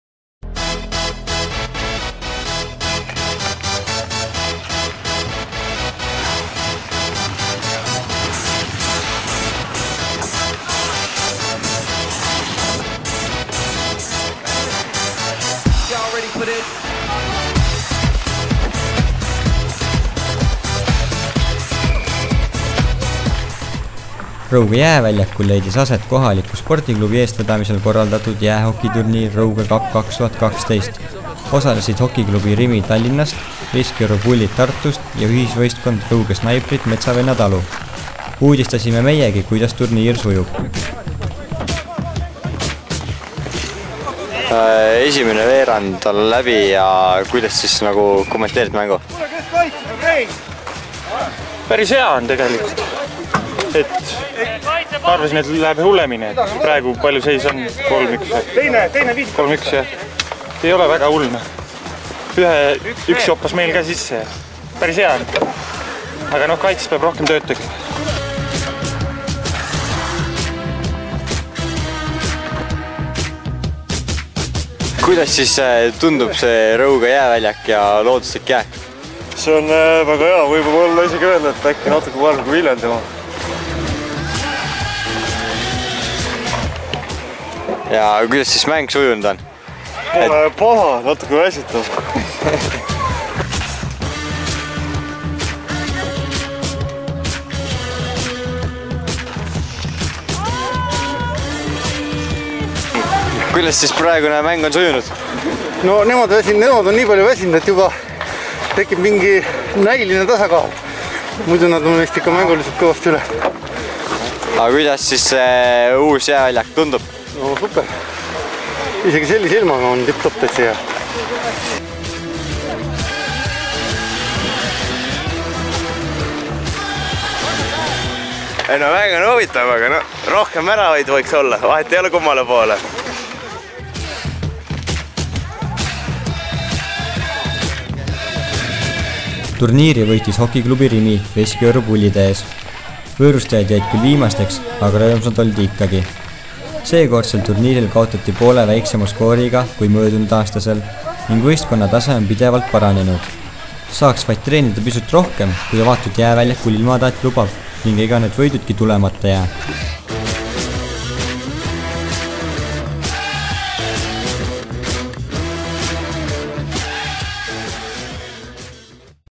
Rõuge värskel jääväljakul peeti esimest hokiturniiri
Hokiturniiril osales kolm meeskonda. Kohalikele Rõuge Snaipritele olid külla tulnud HK Rimi Tallinnast ja HK Veskioru Pullid Tartust. Parimaks osutus HK Rimi, võõrustajad jäid kolmandaks.

Rõuge Televisioon 2012

Reporter: Hektor Glear Vene
Kaamera: Karel Saarna
Montaaž: Karel Saarna
Toimetaja: Jaanus Mark